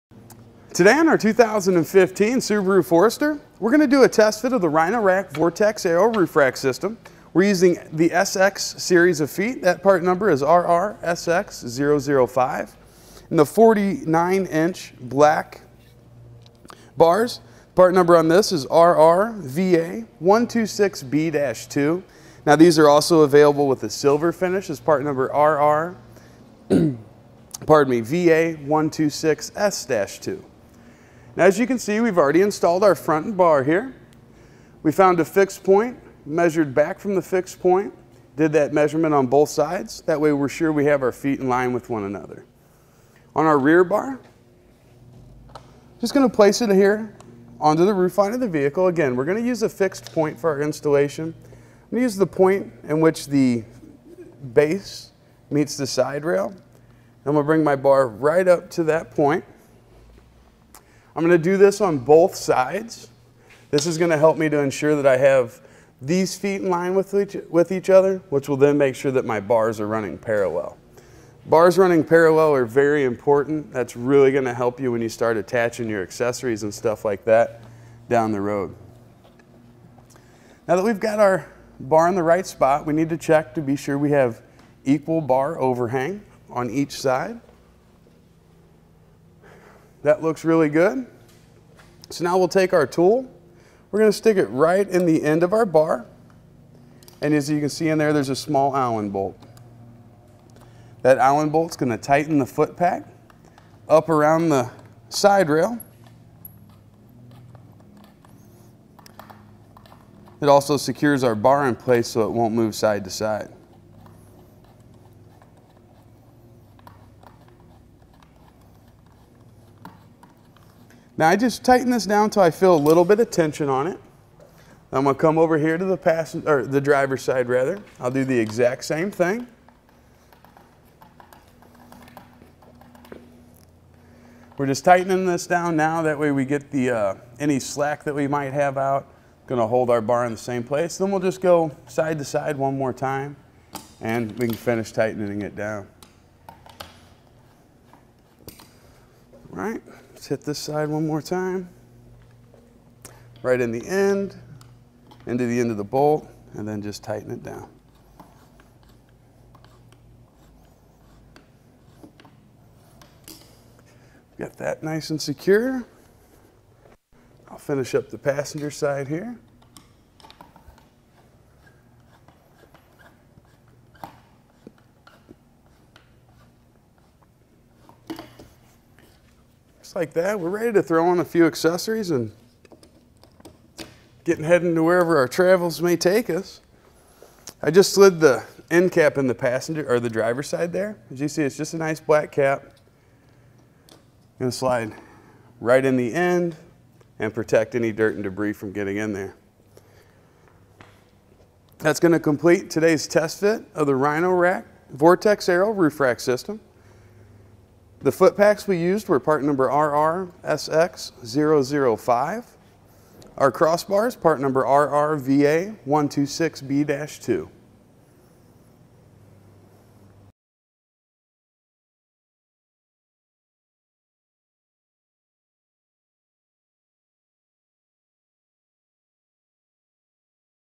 etrailer | Will the Rhino-Rack Vortex Aero Crossbars Fit Your 2015 Subaru Forester?
Today on our 2015 Subaru Forester we're going to do a test fit of the Rhino Rack Vortex Aero roof rack system. We're using the SX series of feet. That part number is RRSX005. The 49" black bars; part number on this is RRVA126B-2. No these are also available with a silver finish as part number RRVA126S-2. Now as you can see we've already installed our front bar here. We found a fixed point, measured back from the fixed point, did that measurement on both sides.

That way we're sure we have our feet in line with one another. On our rear bar just going to place it here onto the roof line of the vehicle. Again, we're going to use a fixed point for our installation. we use the point in which the base meets the side rail. We'll bring my bar right up to that point.

I'm going to do this on both sides. This is going to help me to ensure that I have these feet in line with each other, which will then make sure that my bars are running parallel. Bars running parallel are very important; that's really going to help you when you start attaching your accessories and stuff like that down the road. Now that we've got our bar in the right spot we need to check to be sure we have equal bar overhang on each side. That looks really good, so now we'll take our tool. We're going to stick it right in the end of our bar.

As you can see in there, there's a small Allen bolt. That Allen bolt's going to tighten the foot pack up around the side rail. It also secures our bar in place so it won't move side to side. Now I just tighten this down until I feel a little bit of tension on it. Now I'm going to come over here to the driver side. I'll do that exact same thing.

We're just tightening this down now; that way we get any slack that we might have out. Going to hold our bar in the same place. Then we'll just go side to side one more time and we can finish tightening it down. Let's hit this side one more time. Right in the end into the end of the bolt, and then just tighten it down. We got that nice and secure. I'll finish up the passenger side here. Just like that we're ready to throw on a few accessories and get heading to wherever our travels may take us. I just slide the end cap in the driver side there. As you can see, it's just a nice black cap. Going to slide right in the end and protect any dirt and debris from getting in there. That's going to complete today's test fit of the Rhino Rack Vortex Aero roof rack system. The foot packs we used we're part number RRSX005, our crossbars, part number RRVA126B-2. .